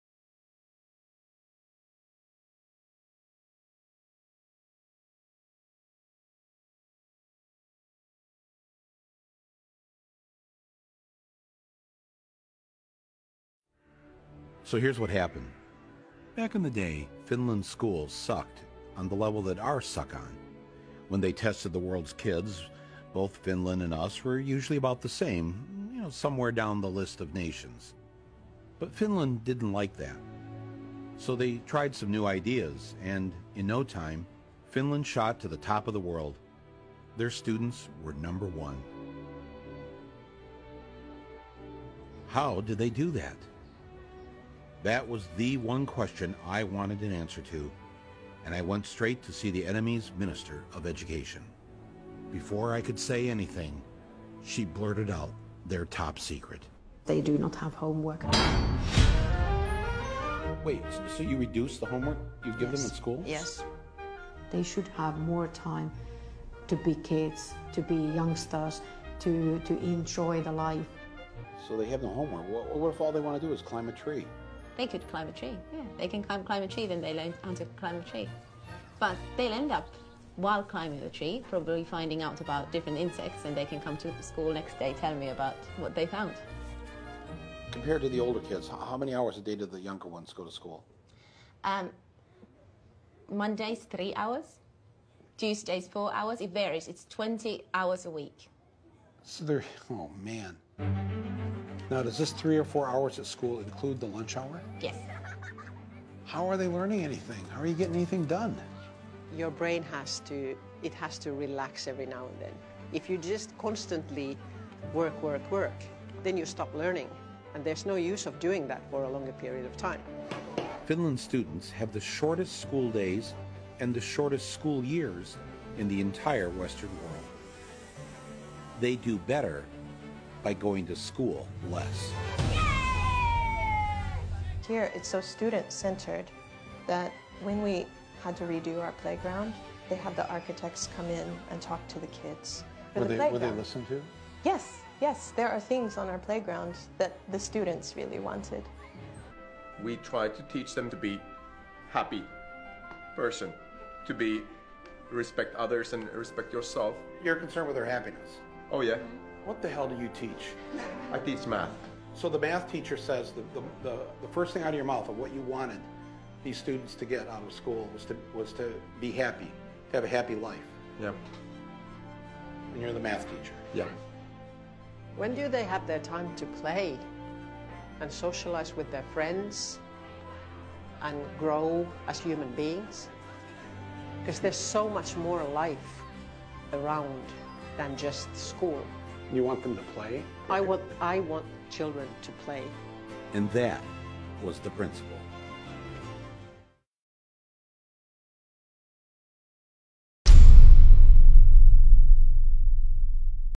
Finland Education Success
Having a child just starting school in Singapore mad me questions our entire education system. I cam across this and want my children to be in schools like this. Now getting to Finland